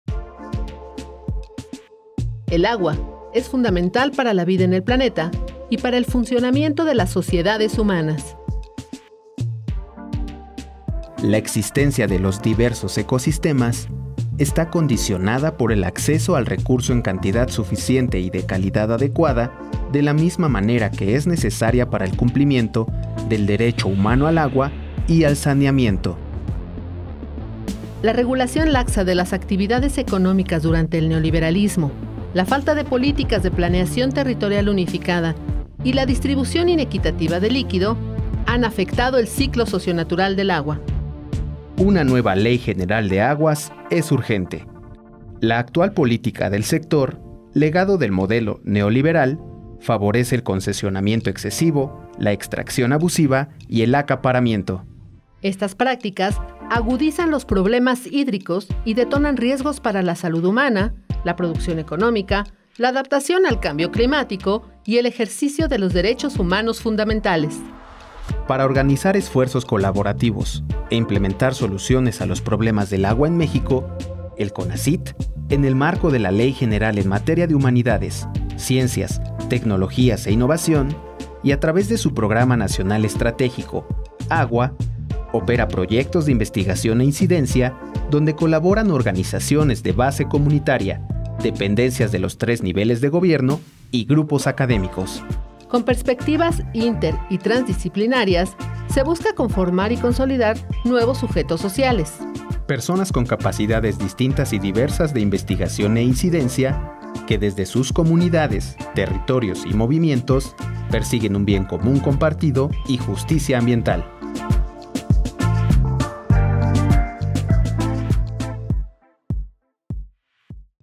Hacia una Ley General de Aguas - Programa Nacional Estratégico "Agua" del Conahcyt
Una nueva Ley General de Aguas es urgente para garantizar el derecho humano al agua y al saneamiento en México.

El Conahcyt, a través del Programa Nacional Estratégico PronacesAgua, coordina los esfuerzos colaborativos de organizaciones de base comunitaria, dependencias de gobierno y grupos académicos, con el objetivo de implementar soluciones a los problemas del agua en nuestro país.